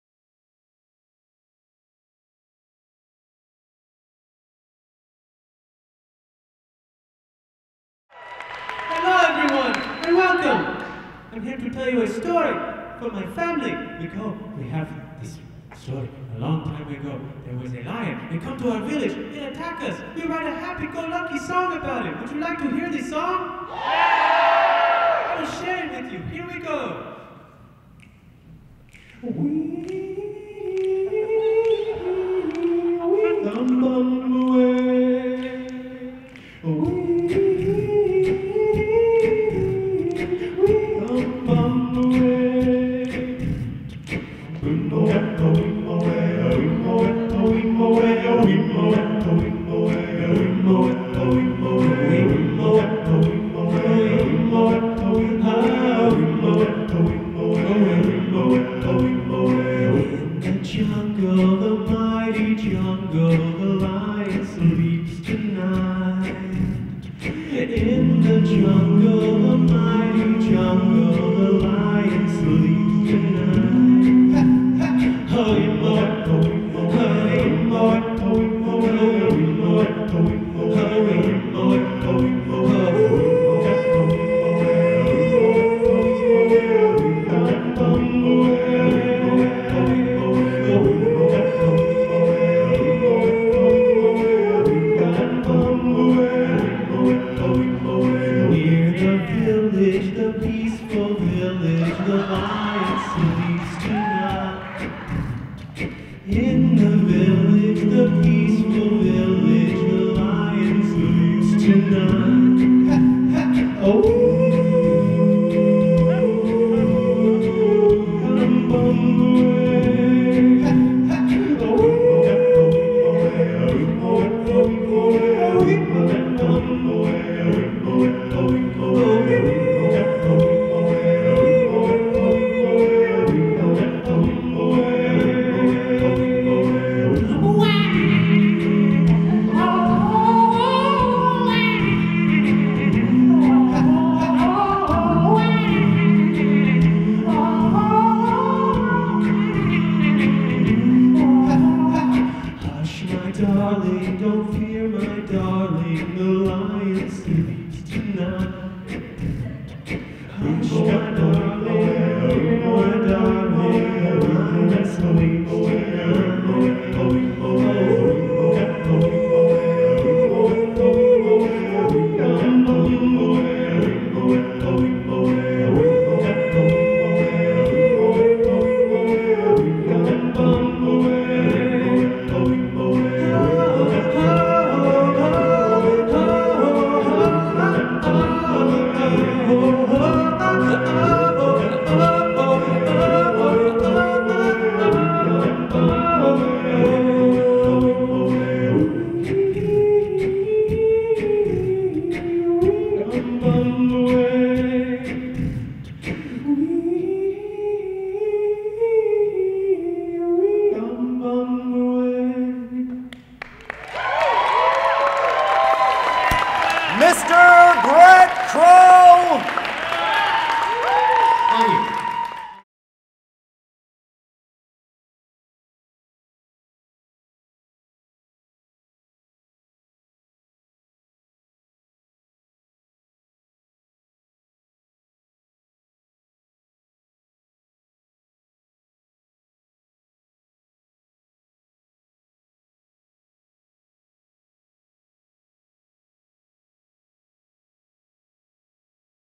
Open Stage 9/23/13 - Bret Crow
Performer: Bret Crow (featuring Glitter the lion)